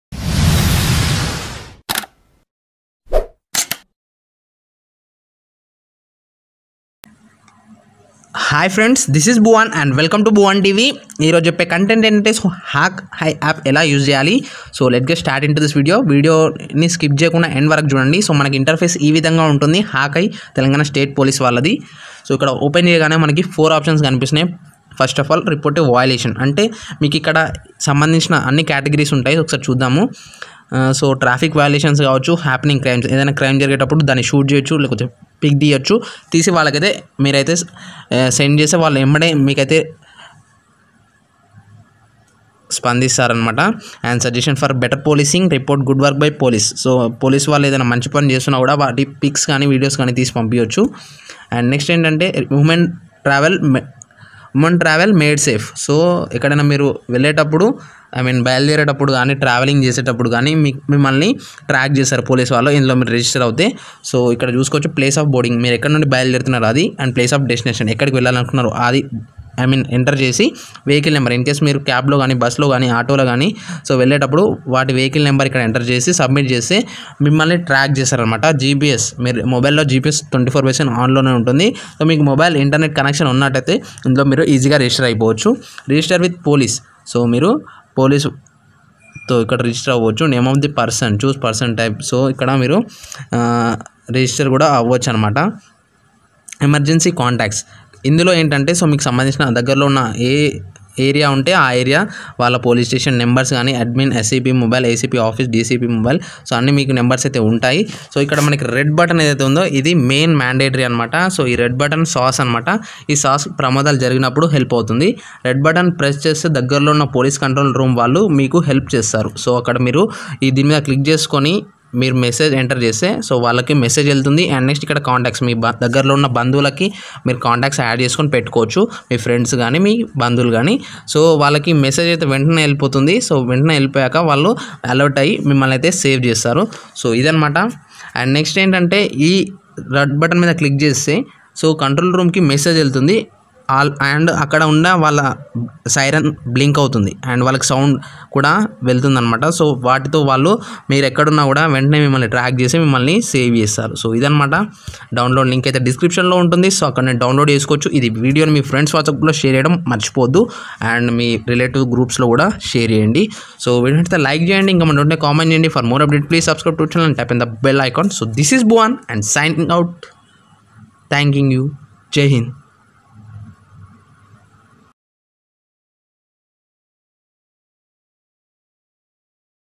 How to use hawk eye Hyderabad police app Telugu|download hawk eye app and uses of hawk eye app
About video: in this video I'm saying about hawk eye app and how we use that app for safety purposes.

steps to register into hawk eye:
1)enter your name
2)enter your mail I'd
3)enter your mobile number
4) confirm your password twice
5) now just login with your phone number and password
that's it